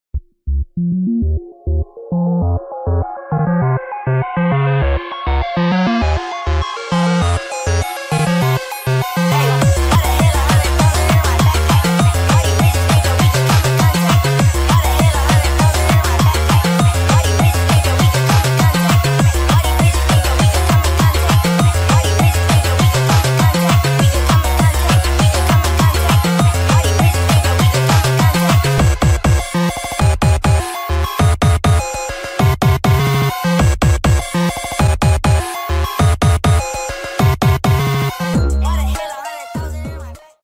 Contact meme📱 // tower heroes animation \\ animation meme \\
Yayyyy a tower heroes animation after days of thinking :D
 Also dont be concerned on why i post many animations is fine really-

Original: deleted?
Inspired: typh
Fps: 15
Apps used: capcut and flipaclip
Character used: byte
This character is not mine! It belongs to a game called tower heroes on roblox!

༼ つ ◕◡◕ ༽つ♥︎